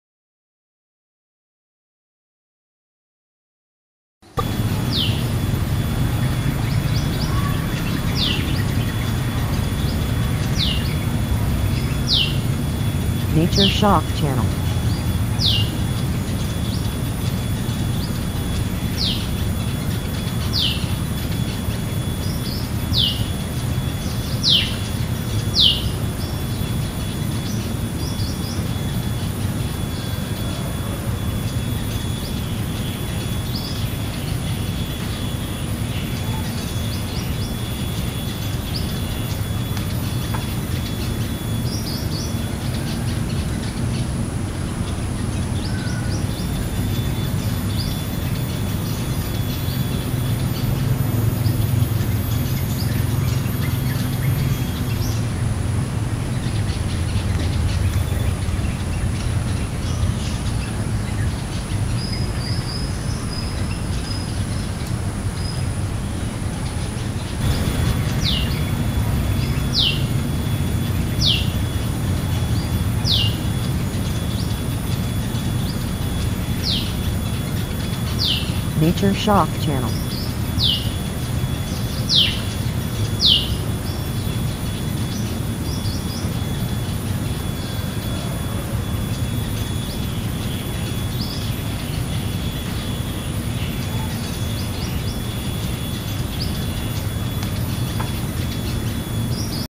Orange Dragonfly Close Up in Nature Shots Video
Dragonflies become one of the beautiful insects that is quite interesting from the body shape and body color. Dragonflies are often found in the yard of the house, in the park, the river, rice fields and shrubs, as well as in many other places. The dragonfly habitat in some areas is already increasingly rare, and not as much habitat dragonflies in previous years.

Many interesting dragonfly color variants and very worthy of being recorded into nature shots photographic works and as a dragonfly video shots. Orange dragonfly is one of the attractive colors of dragonflies, apart from red dragonflies, purple dragonfly, green dragonfly, yellow dragonfly, brown dragonfly, and other dragonfly colors.